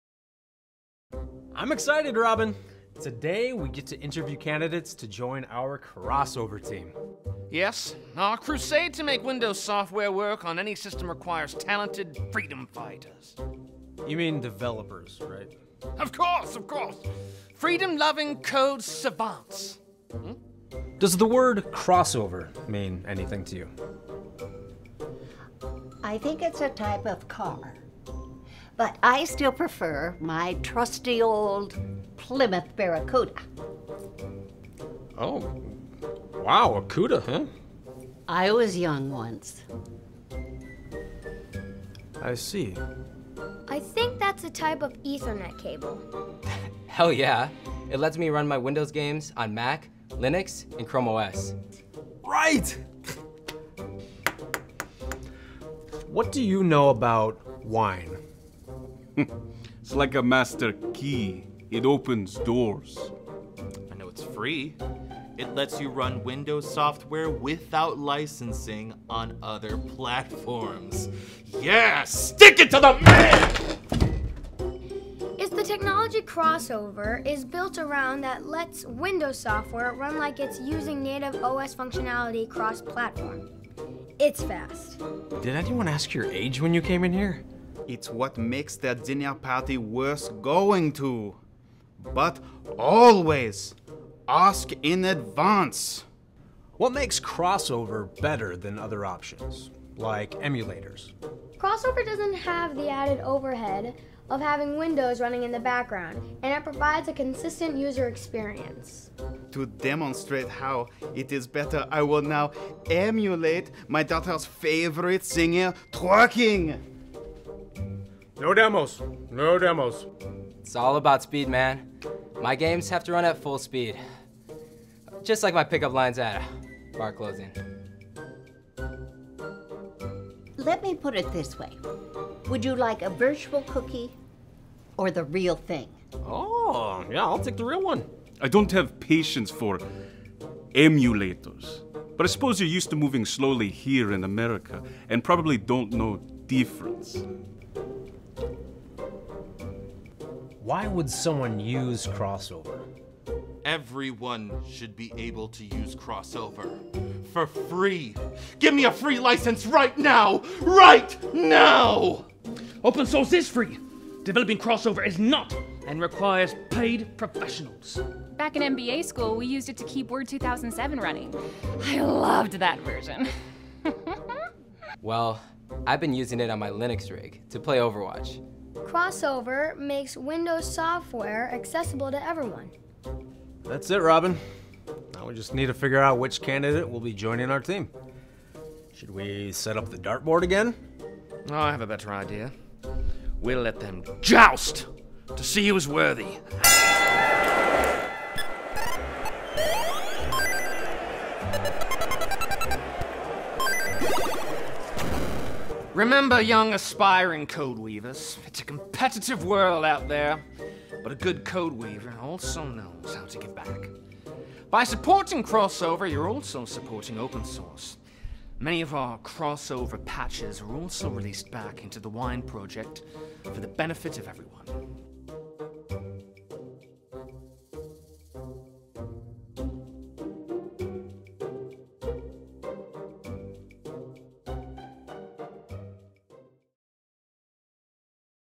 CrossOver™ Explained
We are the inventors of CrossOver™ software. CrossOver is the easiest way to run many Microsoft applications on your Mac, Linux and Chrome OS without a clunky Windows emulator.  CrossOver works differently. It's not an emulator. It does the work of translating Windows commands into Mac, Linux and Chrome OS commands so that you can run Windows software as if they were native.

CrossOver works with all kinds of software - productivity software, utility programs, and games - all with one application.

Faster. Better experience. No license. Simple as that. Starting at $39.95